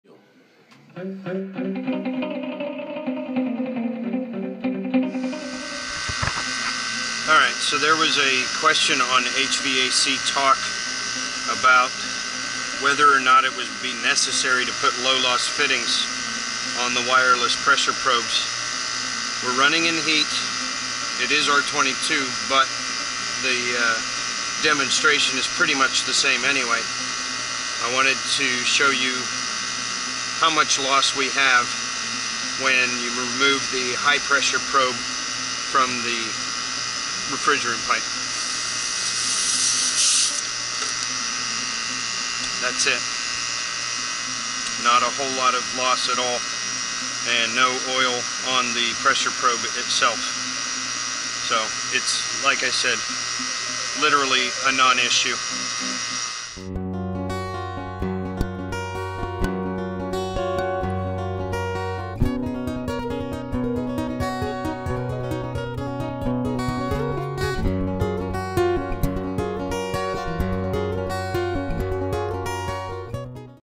HVAC Tools: iManifold iConnect Wireless Pressure Probes No Need For Low-Loss Fittings
Save up to 30% when you sign up with this link to Housecall Pro!

There has been some question about whether or not a low-loss fitting is needed with the pressure probes used with iManifold and iConnect.